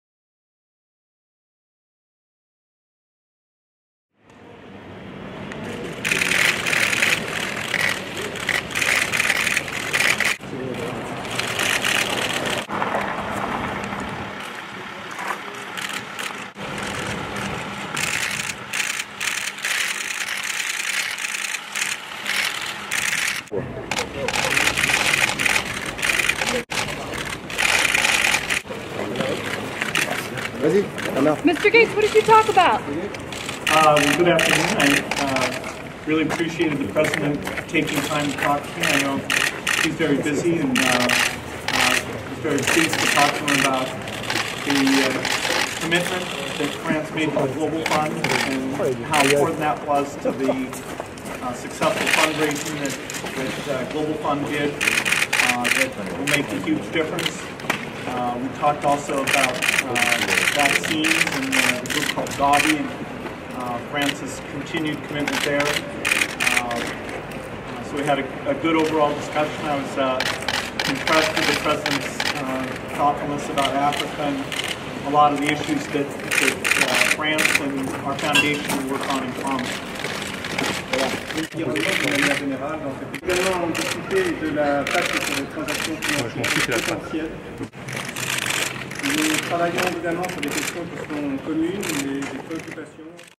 François Hollande greets Bill Gates in Paris
French President Francois Hollande greets Bill Gates, the co-Founder of the Microsoft company and co-Founder of the Bill and Melinda Gates Fondation, prior to a meeting at the Elysee Presidential Palace, in Paris, France, on April 1, 2014.

Le président français Francois Hollande reçoit Bill Gates, le co-Fondateur de Microsoft et le co-fondateur de 'the Bill and Melinda Gates Fondation', pour un rendez-vous au palais de l'Elysée à Paris, France, le 01 avril 2014.